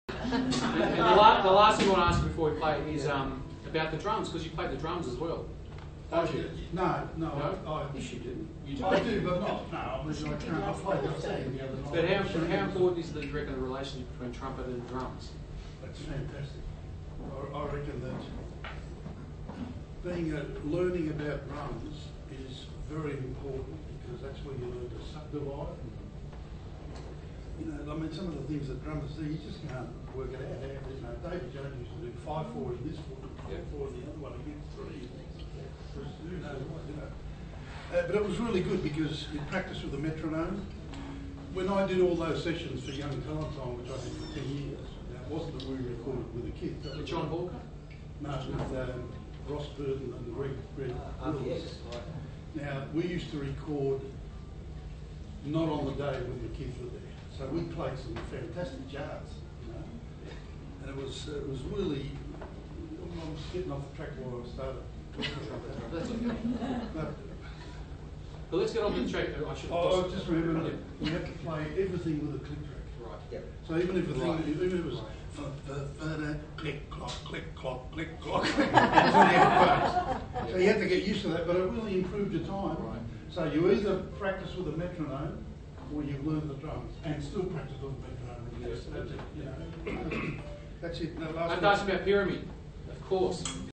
Bob Venier
Australian trumpet legend Bob Venier talks about the value for horn players of learning drums (part of interview with Gianni Marinucci at the Bob Venier tribute night featuring Bob, with special guests Gianni Marinucci, Tony Floyd, Frank DiSario and James Sherlock.